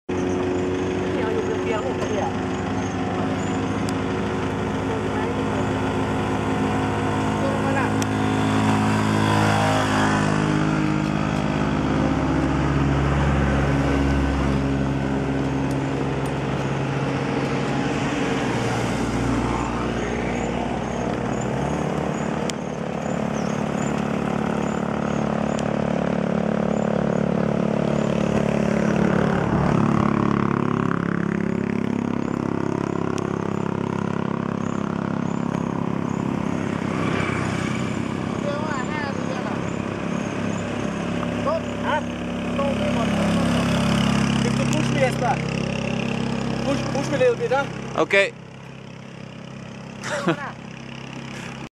Push me a little bit
Doi Inthanon Race 2013. Last steep part.